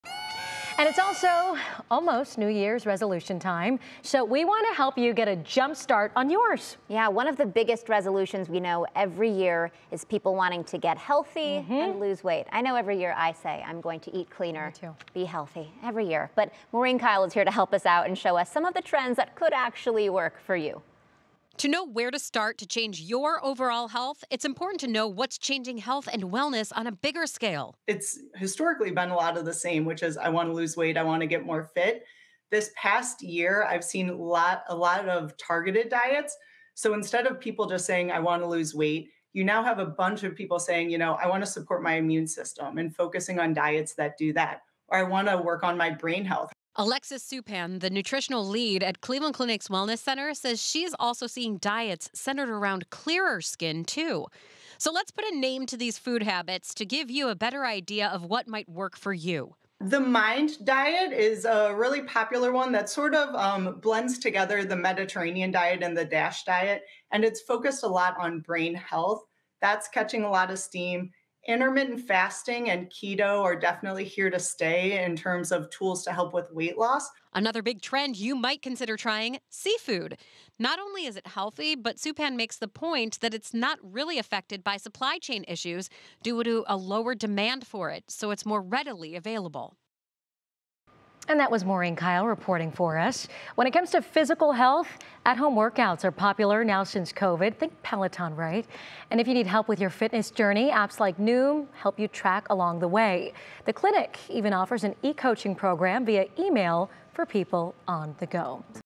Improving your health: Kick-start your New Year's resolution
As 2022 grows near, it is nearly resolution time. One of the biggest resolutions every year: improve your health and lose weight.

We went to the experts to find out what health trends are popular going into the new year to help you kick start your resolution. To know where to start to change *your* overall health, it's important to know what's changing health and wellness on a bigger scale.

"It's historically been more of the same, which is, 'I want to lose weight, I want to get more fit,'" Alexis Supan, the Nutritional Lead at Cleveland Clinic's Wellness Institute, said. "This past year, I've seen a lot of targeted diets. So instead of just people saying 'I want to lose weight,' you have a bunch of people saying, 'I want to support my immune system' and focusing on diets do that, or, 'I want to support my brain health.'

When it comes to physical health, at-home workouts are really popular since COVID (think Peloton). And if you need help with your fitness journey, apps like NOOM help you track along the way. The Clinic even offers e-coaching programs via e-mail for people on the go.